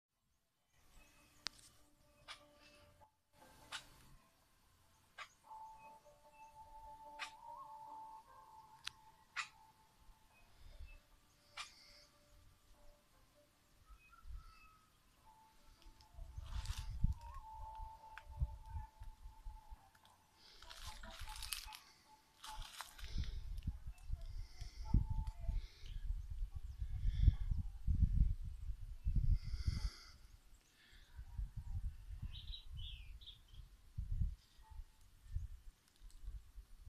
Guapa o Muchilero
Psarocolius decumanus ( Llamado en Dolores Tolima  Guapa o Mochilero)

Guapa o Muchilero (Aves de nuestras fincas cafeteras): Los machos miden alrededor de 43 cm y las hembras 33 cm. Tienen pico grande y blanco hueso o amarillo claro y ojos azules. Principalmente negro lustroso, la rabadilla e infracaudales de color castaño. Tienen una cresta de plumas negras filiformes y largas a menudo conspicuas. Las plumas de la cola son de color negro y el resto amarillo brillante.
Historia Natural: Se observa en parejas o grupos forrajeando en la parte alta del dosel, en ocasiones con bandadas mixtas. Se alimentan de frutos y néctar. Construyen sus nidos en ramas de árboles altos, en forma de bolsa colgante, forman colonias pequeñas con un macho dominante y otros subordinados y tienen apareamiento promiscuo.